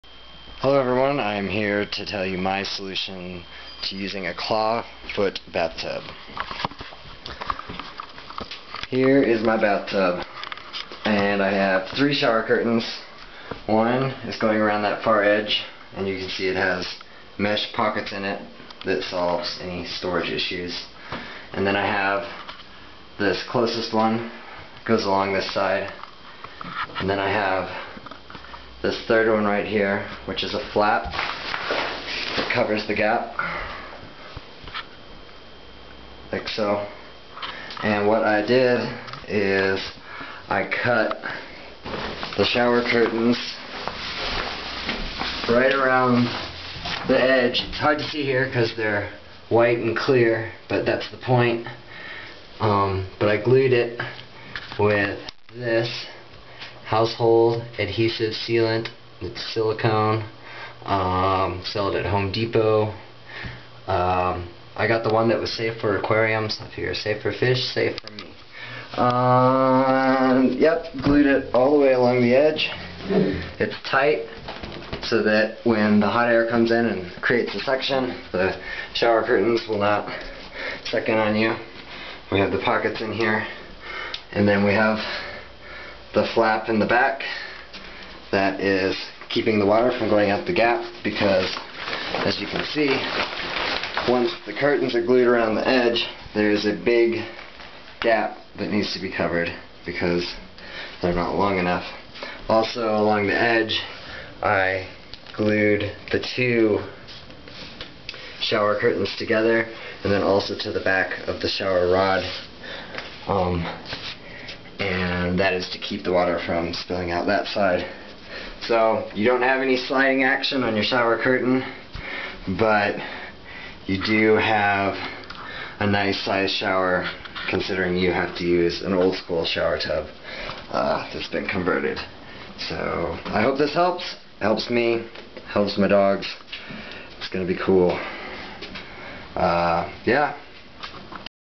clawfoot tub shower curtain liner solution
here is what i did to prevent to curtains from attacking me while i shower